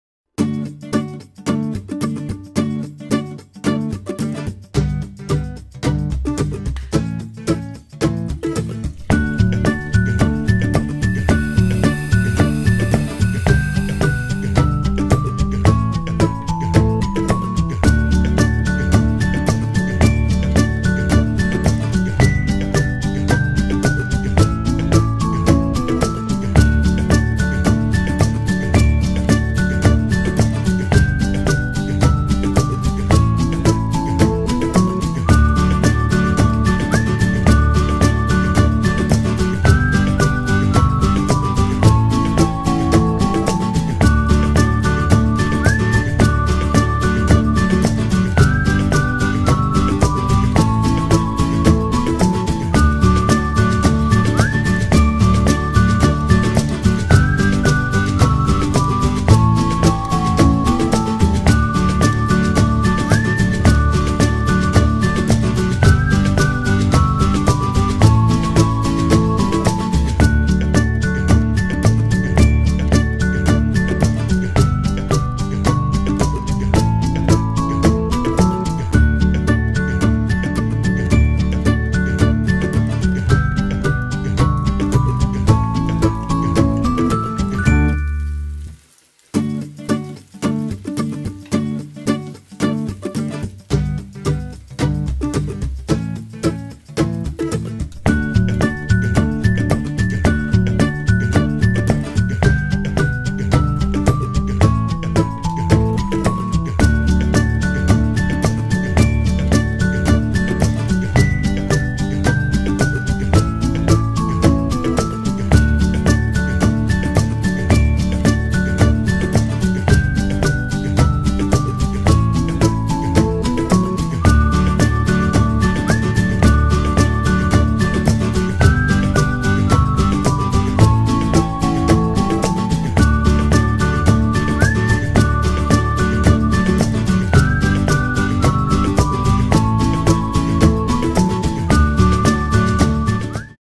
Receta tortilla de patatas cocidas (Mas Sana)
Como hacer una tortilla de patatas cocidas fácil y rápido. Y ademas muy sana!! Receta de tortilla de patatas